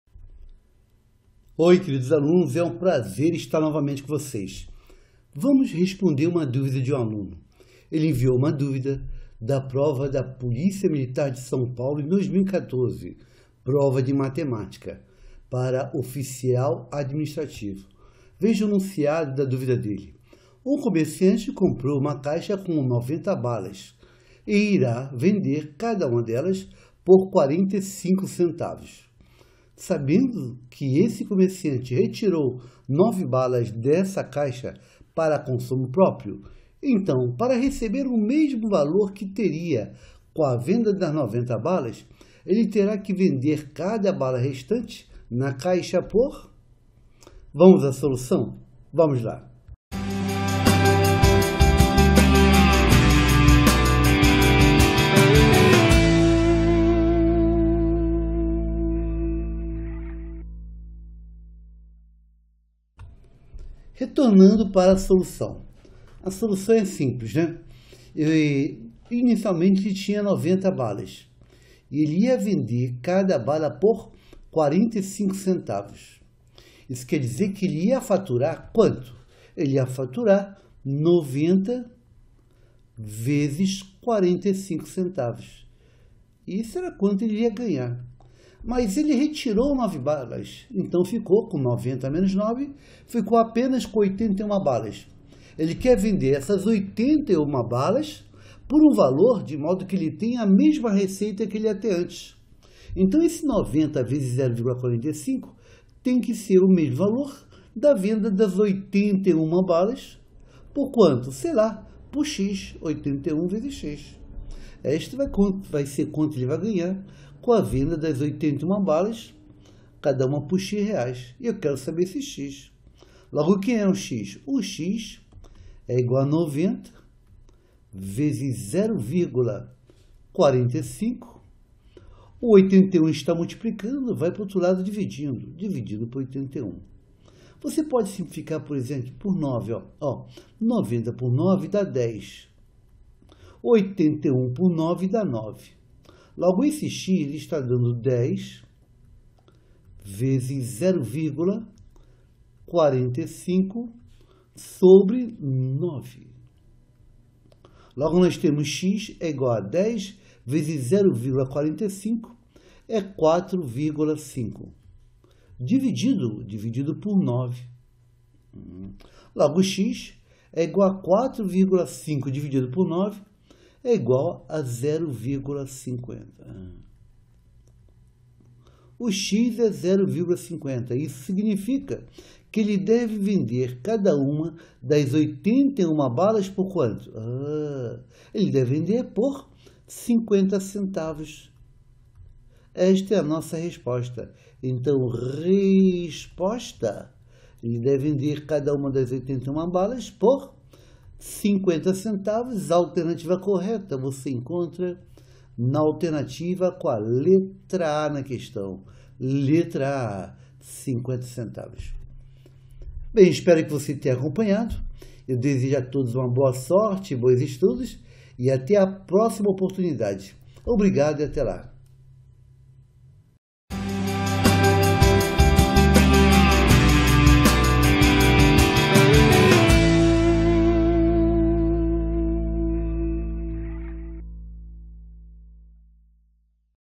Concurso para Oficial Administrativo PMSP MATEMÁTICA VUNESP 2014 RESOLVIDA
(2014 - VUNESP - Oficial Administrativo – PMSP) Um comerciante comprou uma caixa com 90 balas e irá vender cada uma delas por R$ 0,45. Sabendo que esse comerciante retirou 9 balas dessa caixa para consumo próprio, então, para receber o mesmo valor que teria com a venda das 90 balas, ele terá que vender cada bala restante na caixa por:
Resposta: A
Visite o site do professor Joselias no link www,professorjoselias.com.br